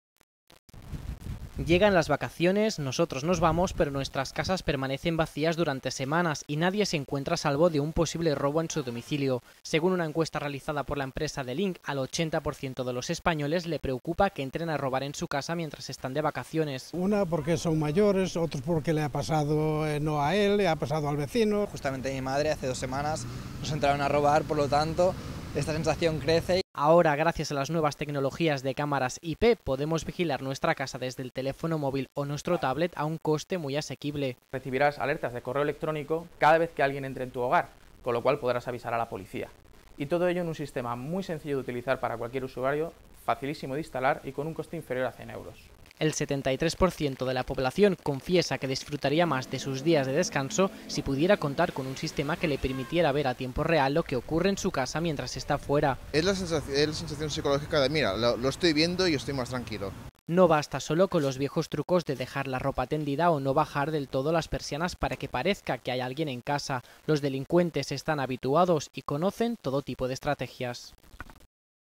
Encuesta Seguridad y Videovigilancia en Vacaciones.mov
Encuesta sobre seguridad del hogar en Vacaciones realizado por la empresa D-Link. Videovigilancia desde móviles iPad, iPhone, Android, alertas de robo por correo electrónico